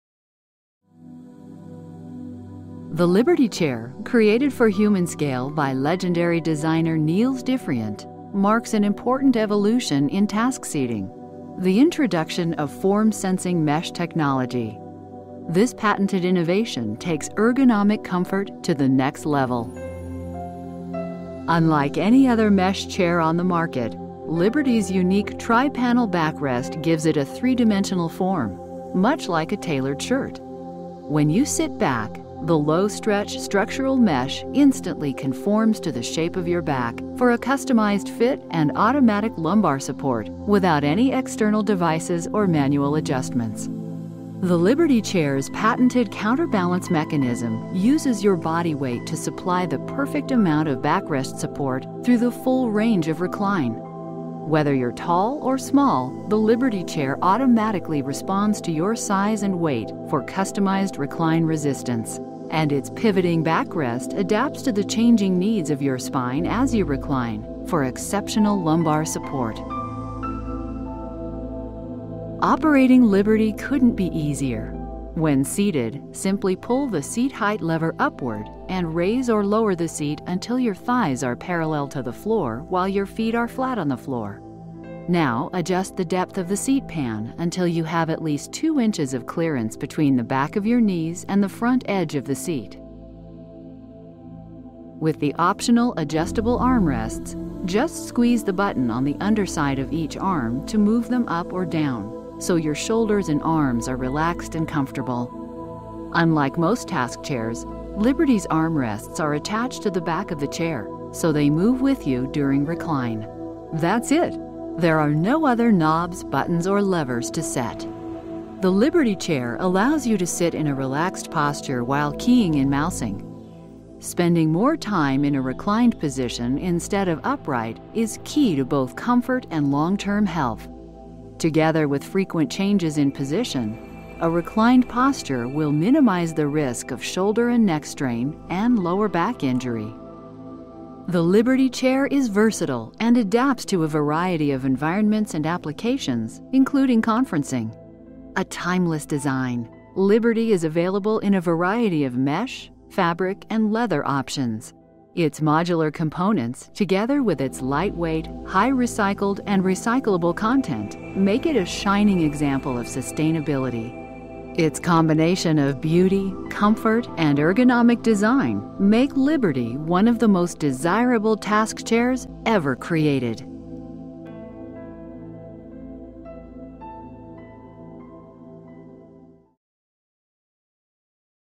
Humanscale Liberty Chair Overview
The Humanscale Liberty Chair is all about uncomplicated, ergonomic features. With a flexing mesh back, comfy gel seat, and intuitive adjustable arms, the Liberty Chair is a top choice for offices or homes anywhere. You can customize your own Liberty Chair at SmartFurniture.com.